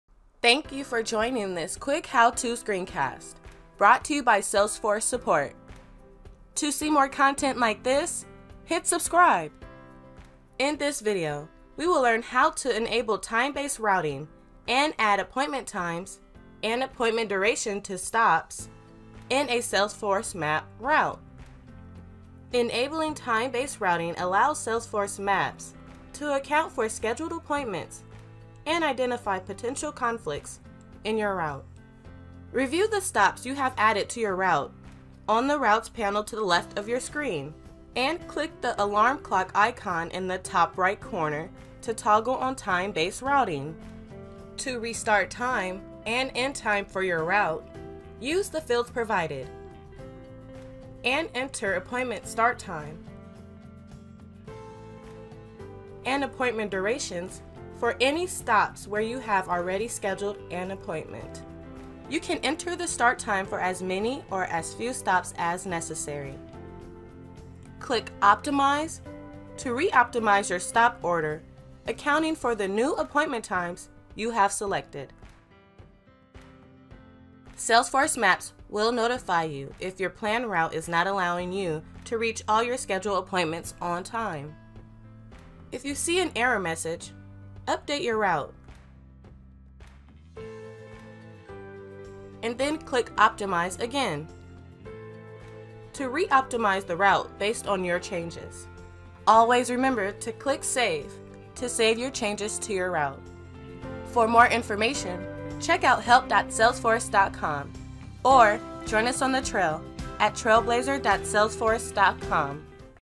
Use Time Based Routing in Salesforce Maps
Watch this video to learn how to enable time based routing and add appointment times and durations to stops in Salesforce Maps Route.